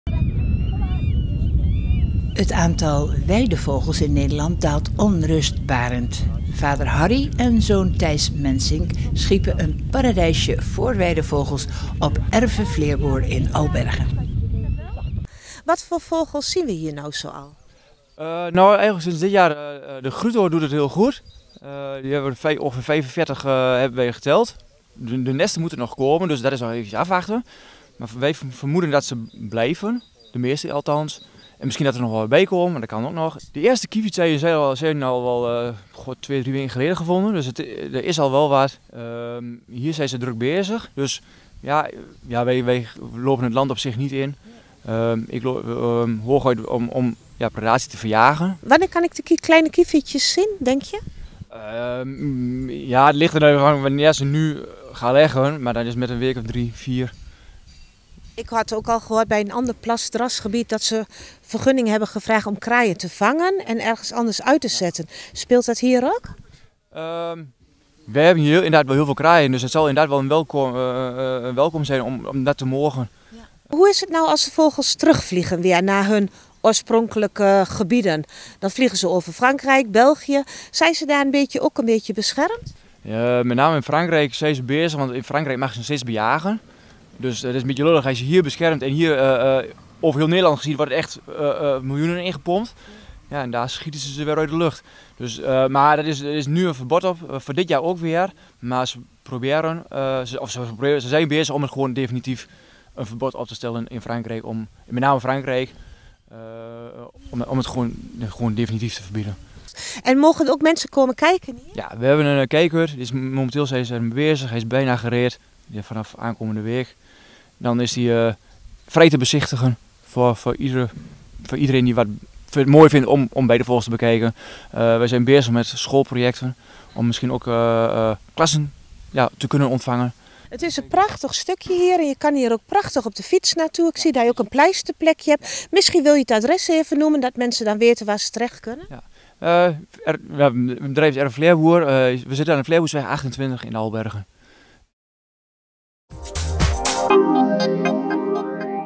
Erve Vleerboer bekommert zich om weidevogels
Vader Harrie en zoon Thijs Mensink schiepen op hun land, aan de Vleerboersweg in Albergen  een paradijsje voor de weidevogels.
Door een perceel plas dras te maken zorgden ze er voor dat er nu al veel weidevogels aanwezig zijn.
Het is duidelijk dat dergelijke gebieden in een grote behoefte voorzien. Tot nu toe telden ze er al 45 Grutto's, zes paar Kieviten, Tureluurs, Wulp en Scholeksters.